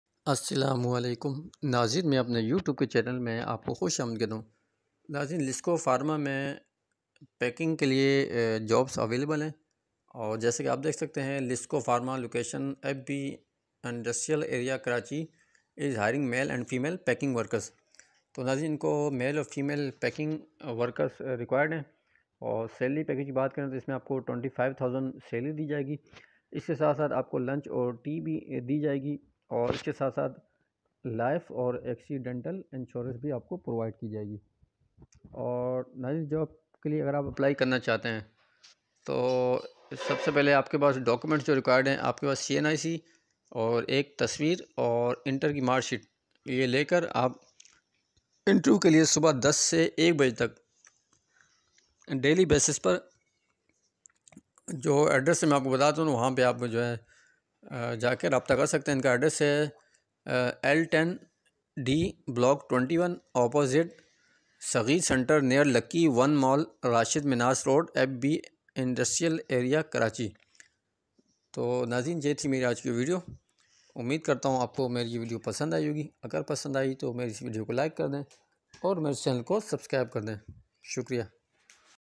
*Lisko Pharma* (Location: FB Industrial Area, Karachi) is hiring Male & Female Packing workers
✅Salary: *25000/-*
✅Lunch and Tea
✅ Life and accidental insurance

Documents required:
*Cnic, 1 photograph & inter marksheet*

Interested candidates can come for *walk in trial* during 10am to 1pm on daily basis (Add: *L10-D, block 21, opposite sagheer center, near lucky one mall, Rasheed minhas road, F.B industrial Area, Karachi*)